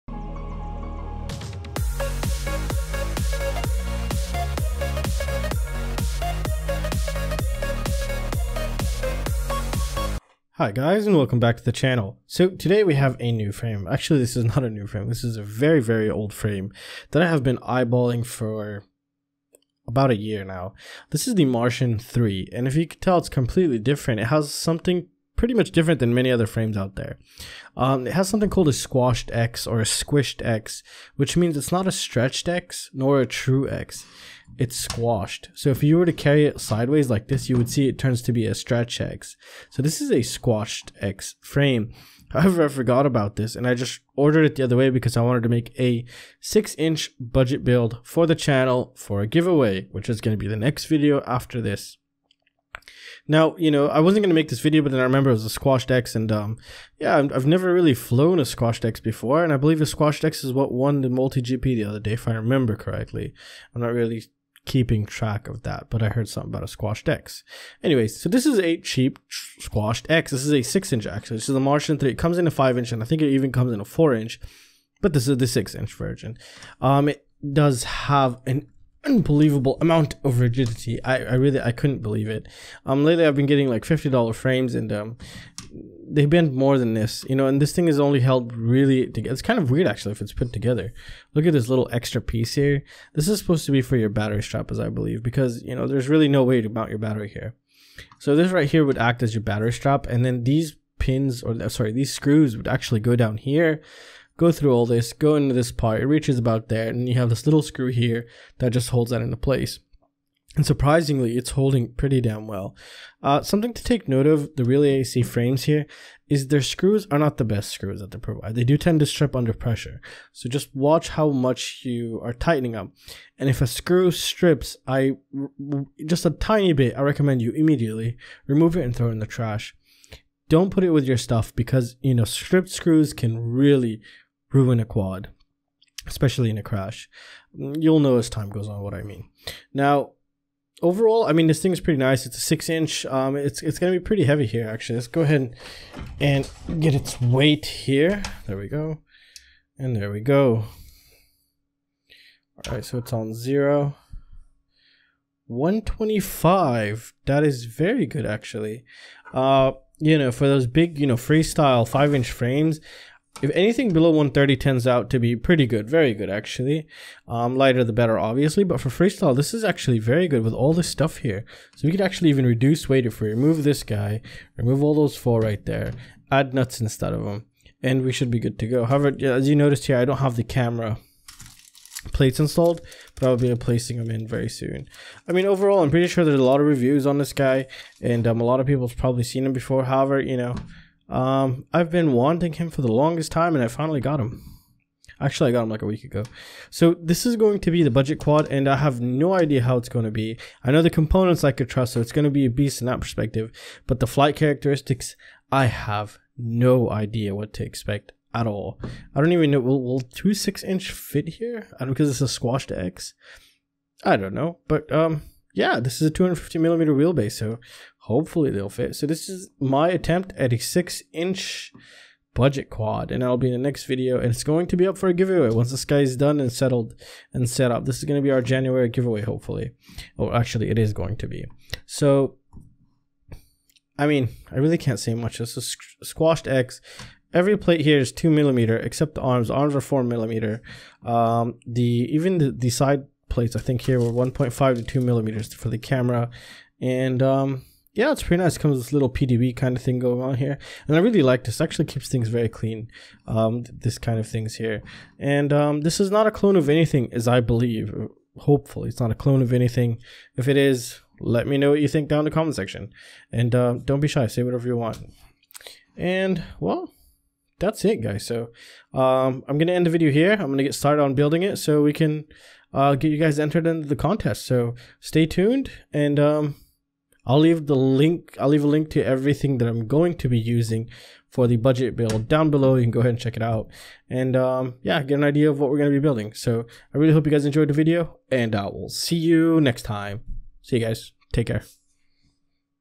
Realacc Martian 3 III 250mm Squashed X // Giveaway
Martian III 250

Components will be Used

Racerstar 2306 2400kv

I highly recommend these boards over 2 months of testing best money can buy

Soldering station